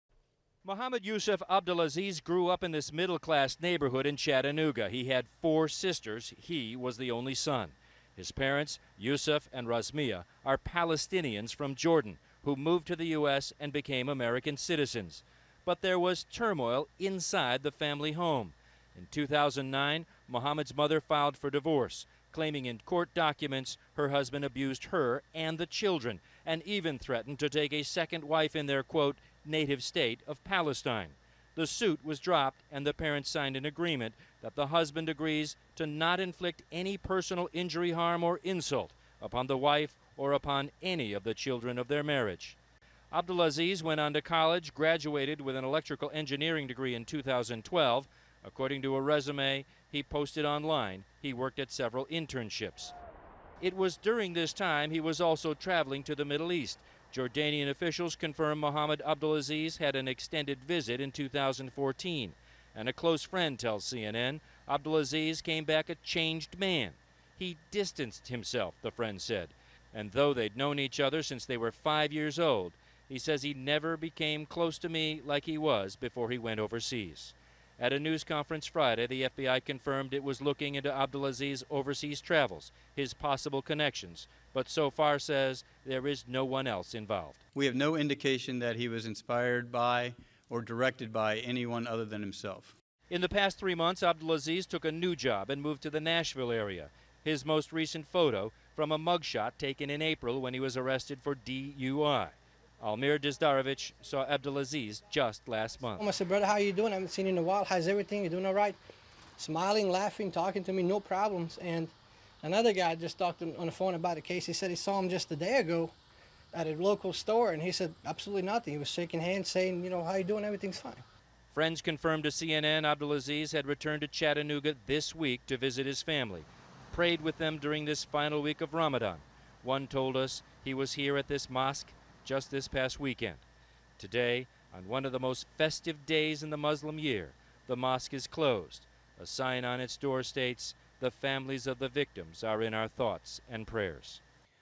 Chattanooga shooter's complicated family life
Drew Griffin looks at court documents that give new insight into the shooter's background and what people who knew him say about a 2012 trip to the Mideast.